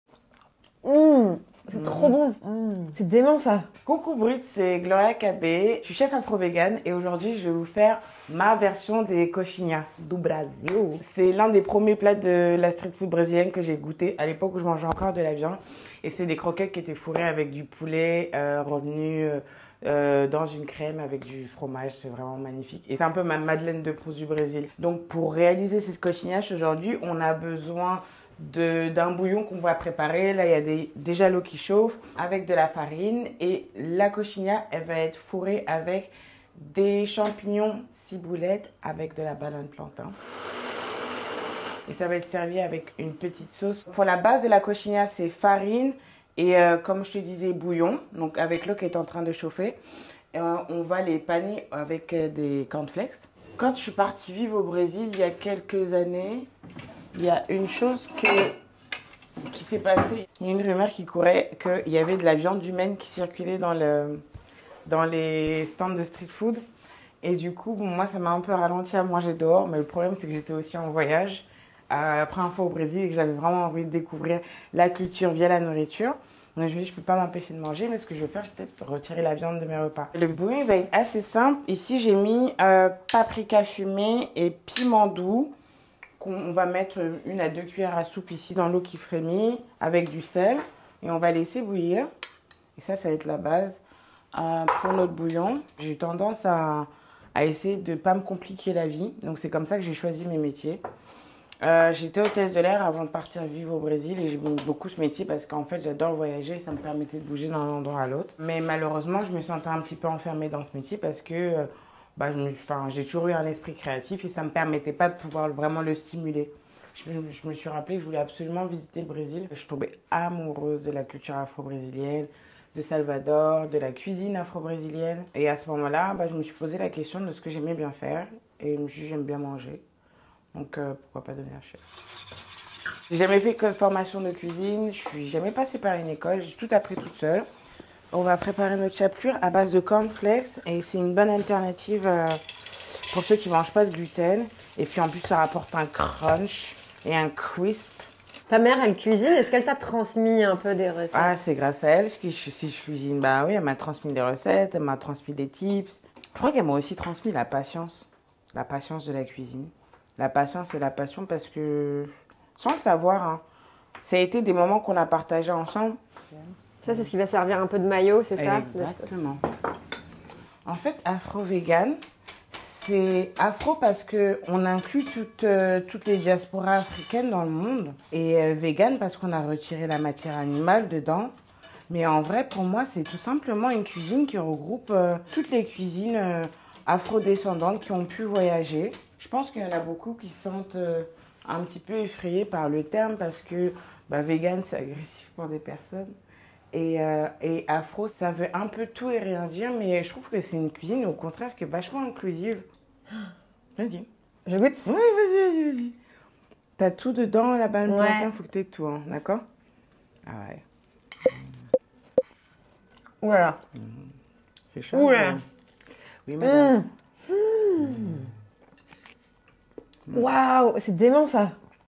How to make Brazilian chicken croquettes
Afro-vegan chef Gloria Kabe shows Brut how to make coxinhas do Brasil...